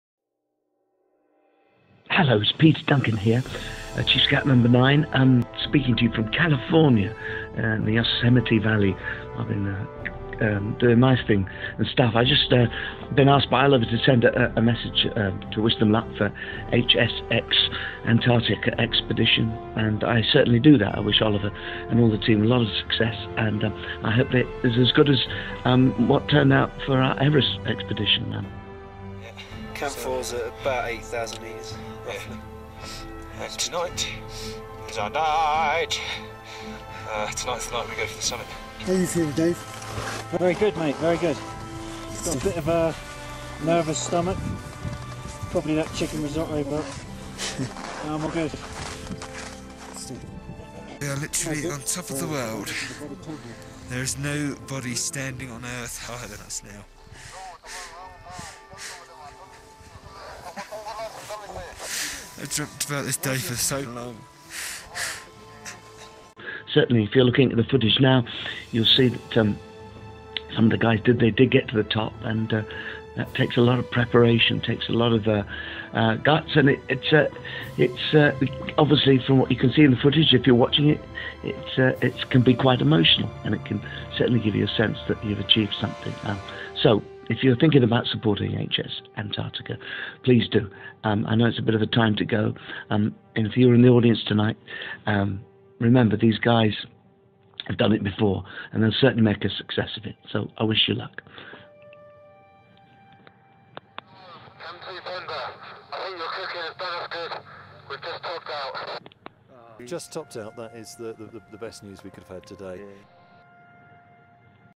Worlds first Scout team to summit Everest
Peter Duncan Chief Scout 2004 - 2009 & Blue Peter Presenter, voice over of the Hampshire Scout Expeditions (HSX) Sky High expedition; The Worlds first Scout team to summit Everest in 2007. Played at the launch of our new expedition to the South Pole in 2018; the Worlds first Scout team to ski to the South Pole.